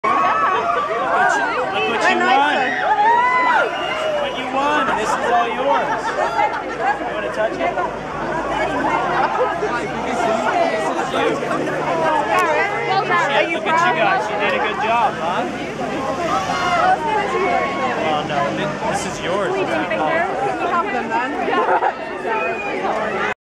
30 Seconds to Mars - Kerrang! Awards - London (09.06.2011)
After the ceremony, Jared offered to touch the awards - TWO big shining "K"s, saying that actually they were ours. Echelon just can't be more happy! :D